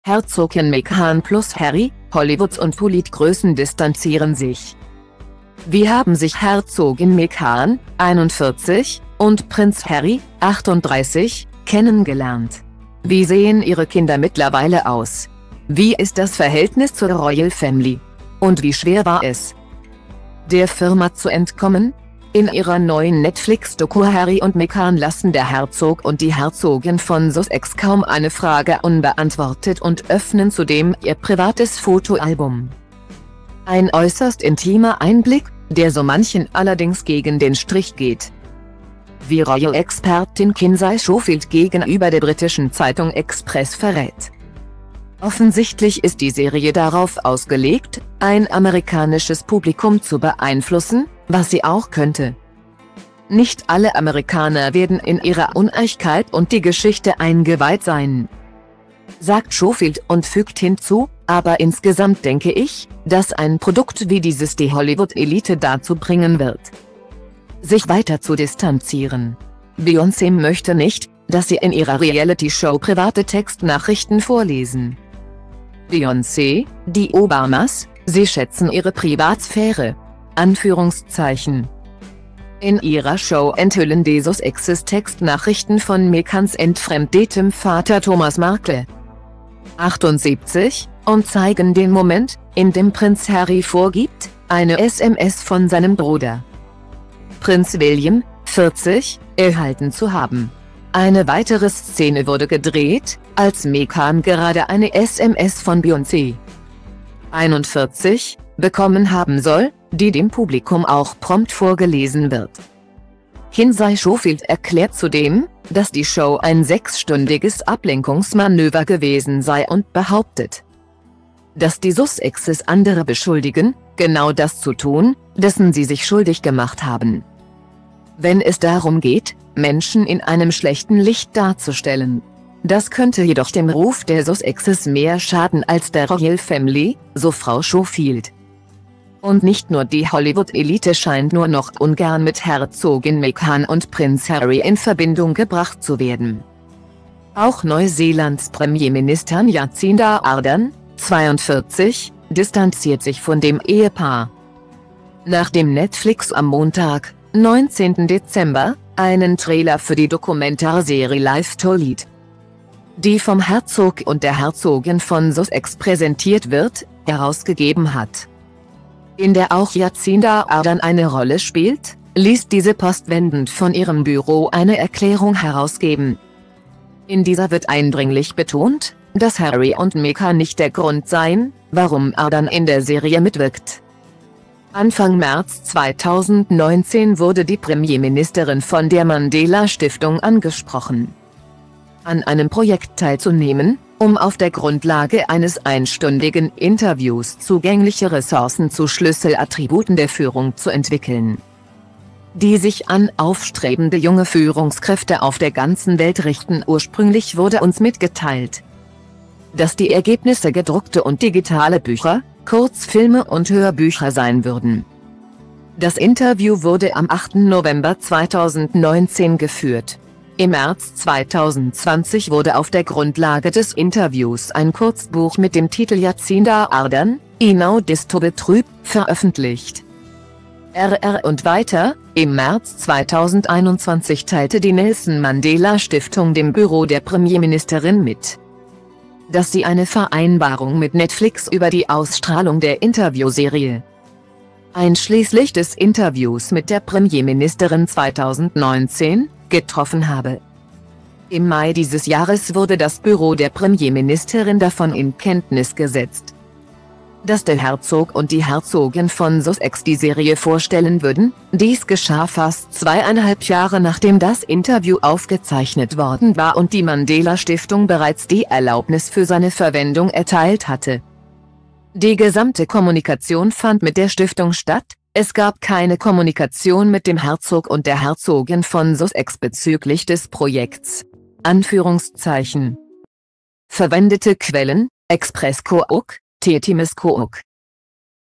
Herzogin Meghan + Harry: Hollywoods und Polit-Größen distanzieren sich
Wie haben sich Herzogin Meghan, 41, und Prinz Harry, 38, kennengelernt? Wie sehen ihre Kinder mittlerweile aus? Wie ist das Verhältnis zur Royal Family? Und wie schwer war es,der "Firma" zu entkommen? In ihrer neuen Netflix-Doku "Harry & Meghan" lassen der Herzog und die Herzogin von Sussex kaum eine Frage unbeantwortet und öffnen zudem ihr privates Fotoalbum.Ein äußerst intimer Einblick, der so manchen allerdings gegen den Strich geht,